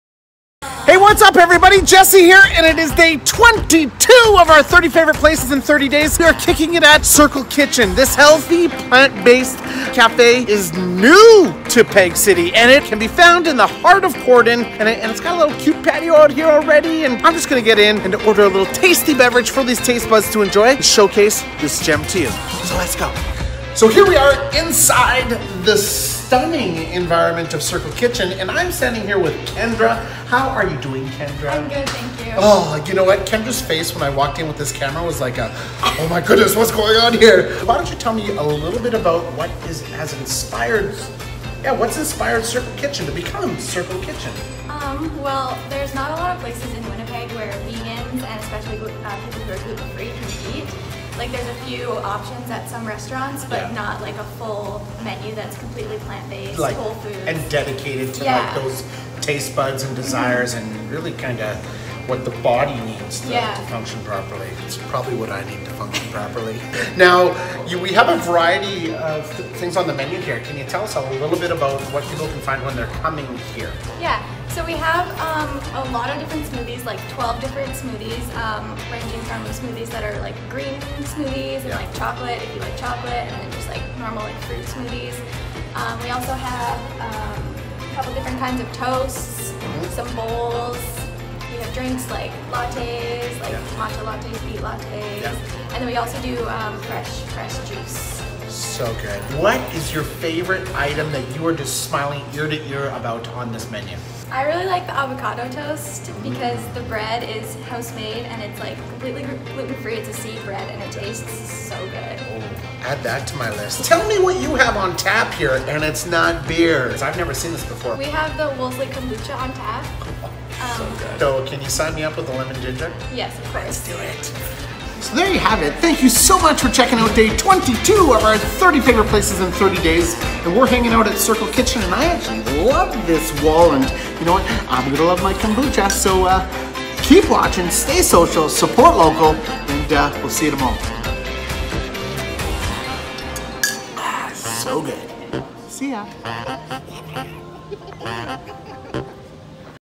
Day 22 - Circle Kitchen - Our 30 Favourite Places in 30 Days - Winnipeg Edition
Day 22 and all it's goodness has arrived! We are all smiles hanging out at Circle Kitchen! Have a watch and find out why!  Vegan goodness and great service can be found here! Located in the heart of Corydon Village, they are a must visit! Plus, I am all smiles to find out they serve Wolseley Kombucha on tap! Be sure to stop in and give them a try!

Have you been yet?  What have you tried?

Please SHARE, LIKE, and ENGAGE on this video. We want to spread the love as much as possible for these places we truly appreciate it!